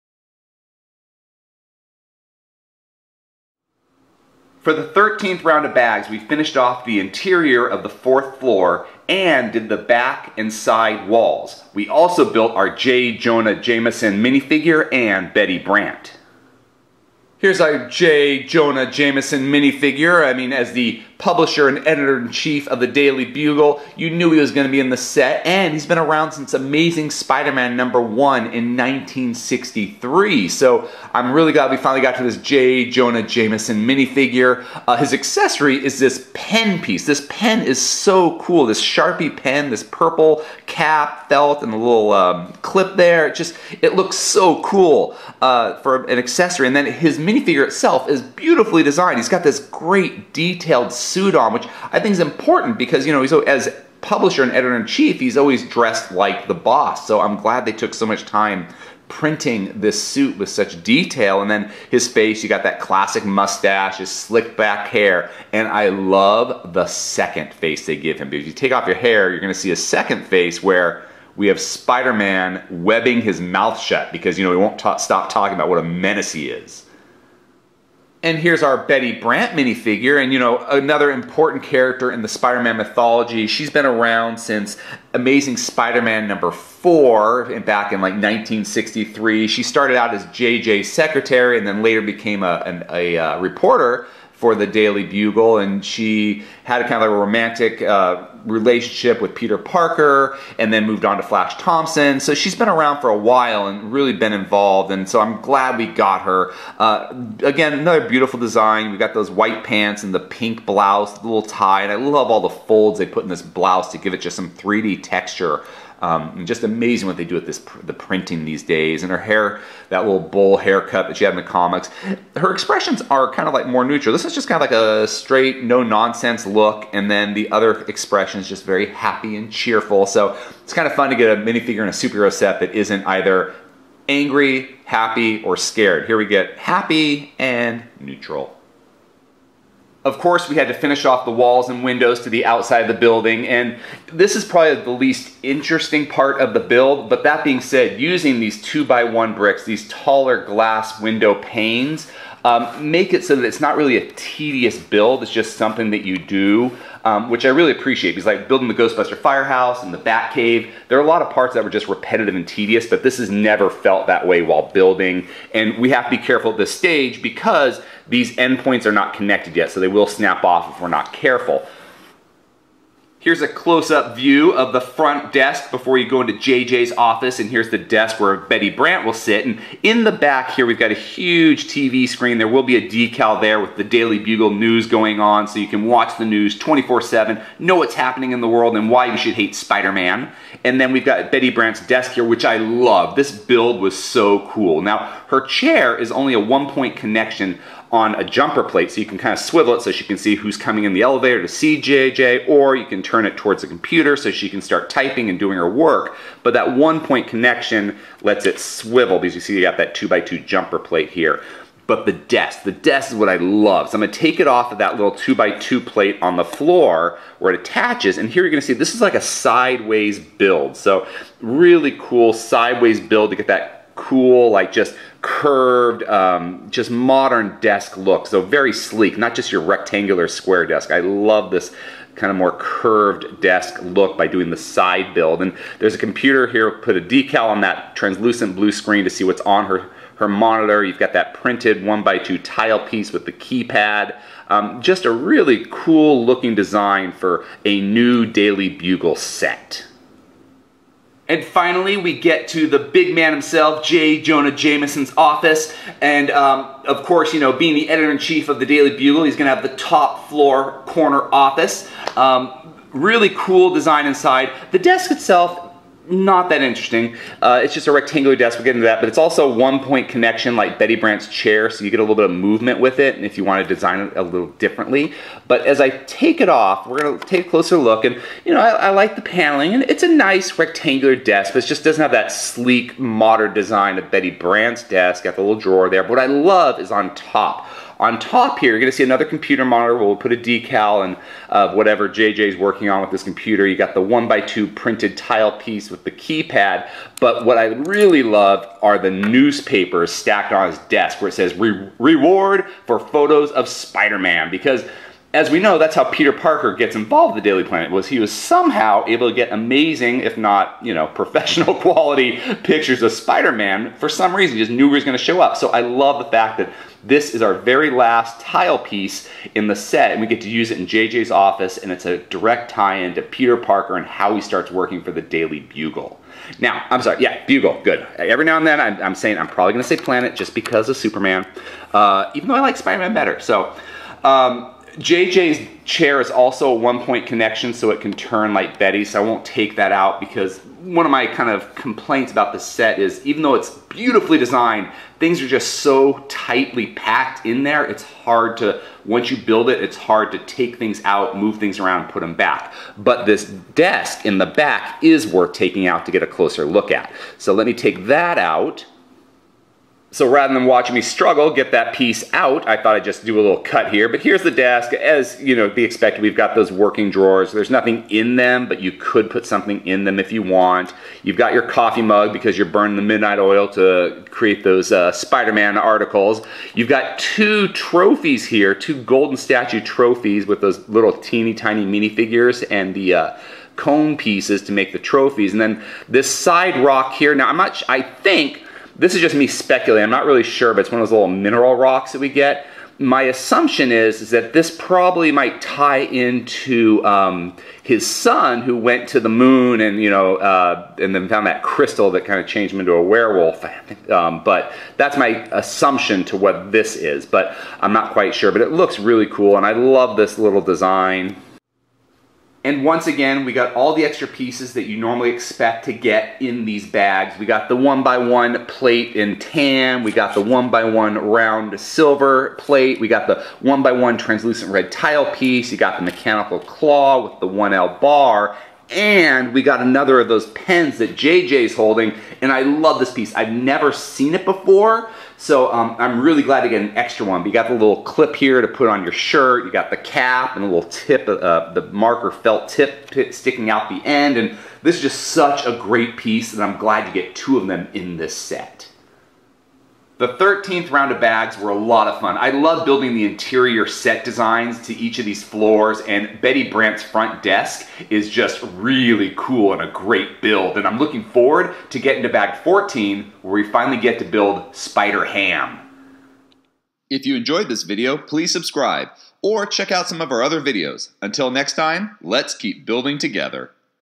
LEGO Spider-Man Daily Bugle 76178 Build Update Bag 13
I show my progress in building the LEGO Marvel Superheroes Spider-Man Daily Bugle 76178. See what is built in the thirteenth bags.